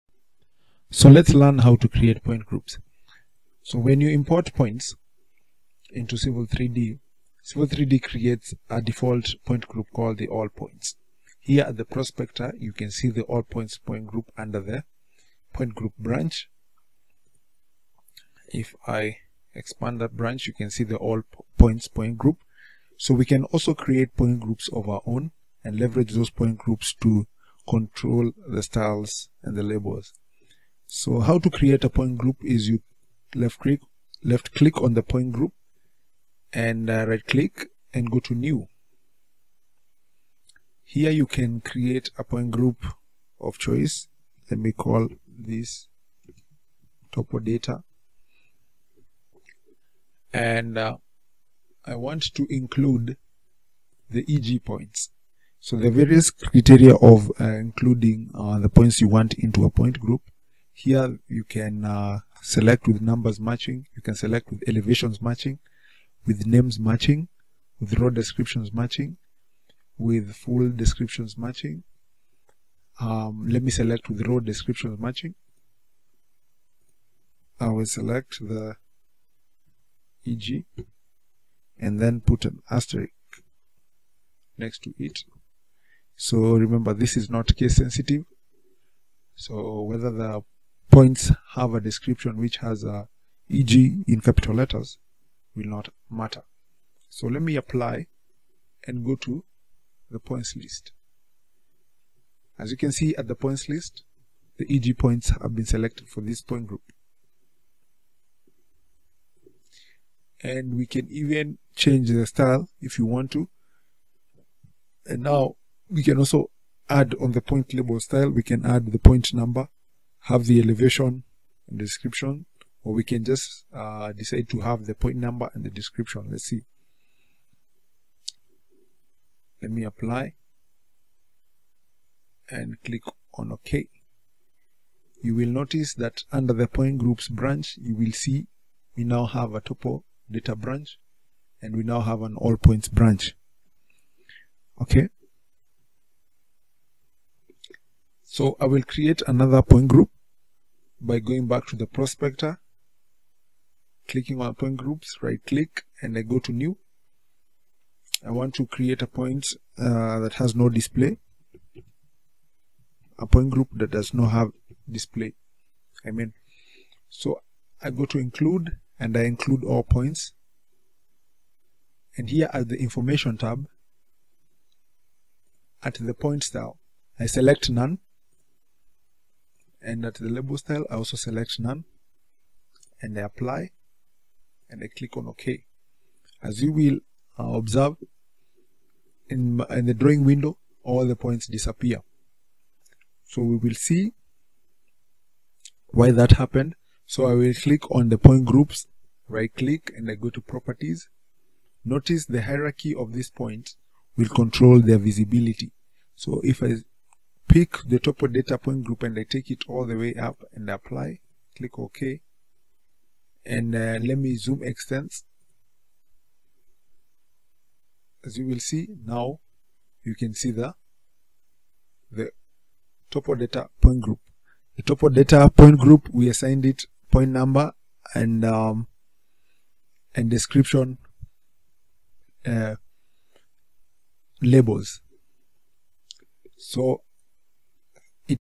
4. Civil 3D Point Groups Tutorial! - How to Use Point Groups in Civil 3D (Video 4/7)
4. Civil 3D Point Groups Tutorial! - How to Use Point Groups in Civil 3D  (Video 4/7). In this session you will learn how to create point groups and to edit them to your preference.

Working with point groups is one of the most powerful techniques you will learn. Do you want to select all your ground shots from within the survey? Make a point group! Want to move up last week’s survey by the blown instrument height difference? Make a
 point group! Want to prevent invert shots from throwing off your surface model? You get it. Use
 a point group!

A point group is a collection of points that results when a criteria-based filter is applied to the
 point data in the drawing. For the filter criteria you can use any point property or combination
of properties such as description, elevation, and point number, or you can select specific points
in the drawing.

You will see that a point group is already created by default in any Civil 3D drawing: the
_All Points group. This group contains all points in the drawing regardless of filter criteria.
This group cannot be renamed or deleted, nor can it have its properties modified to exclude any
points. Therefore, you should create point groups for collections of points you might want to
 separate from others.